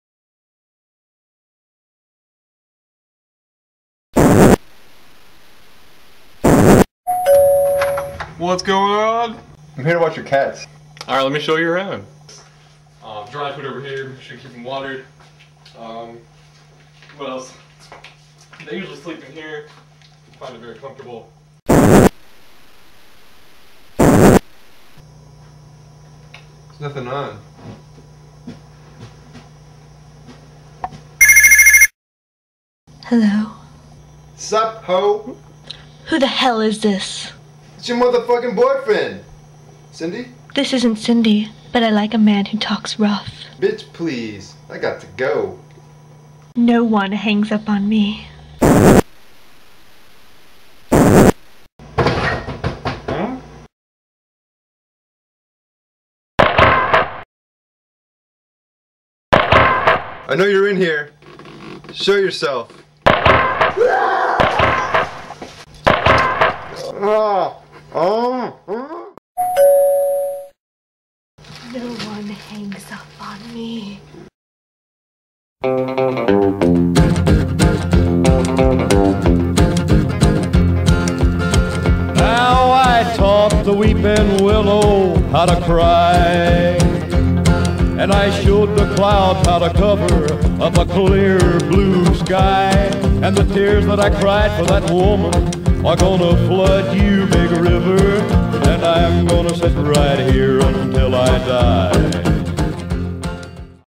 When You Call A Stranger Trailer
Trailer for the fictional movie When You Call A Stranger.  If you like this also see Terminator 4 and Soulemite.